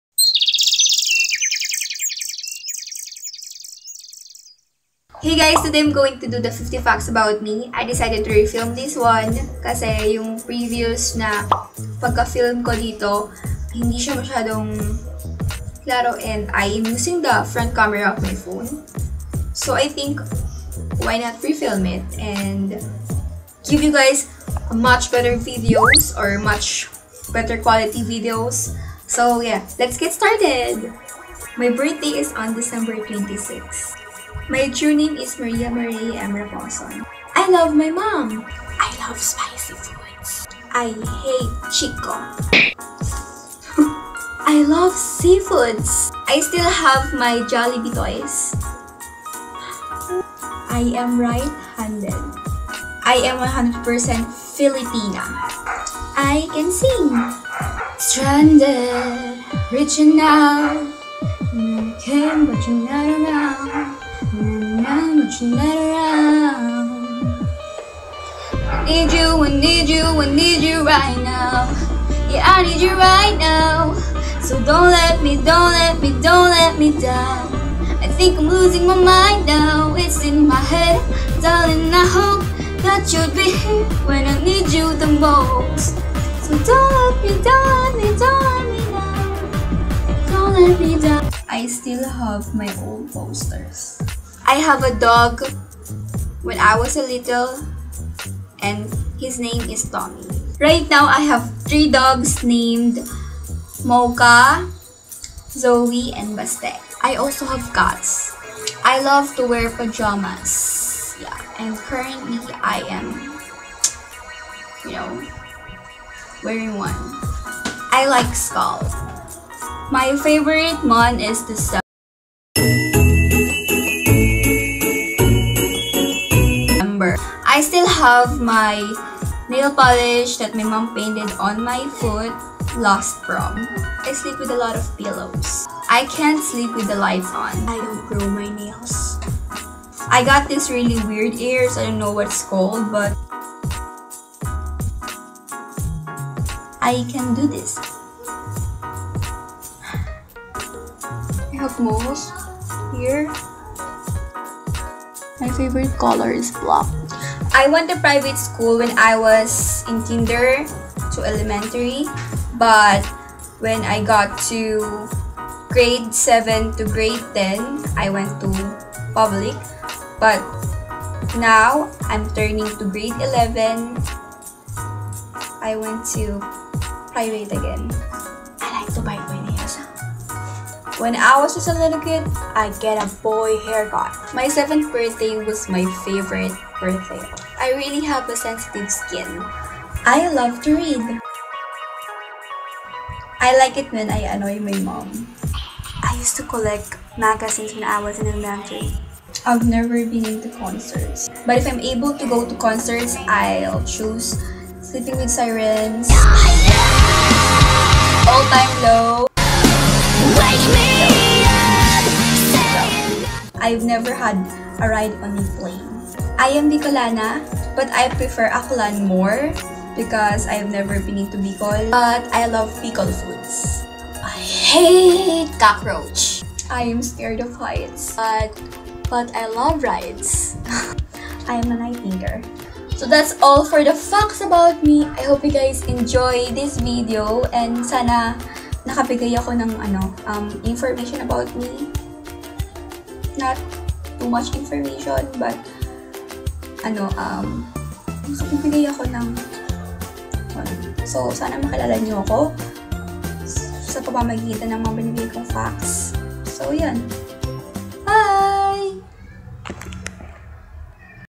50 FACTS ABOUT ME TAG | Maria R.
Hello everyone!! Todahhh I'm gonna do the 50 facts about me tag!

I TAG YOU!! YESS YOU!!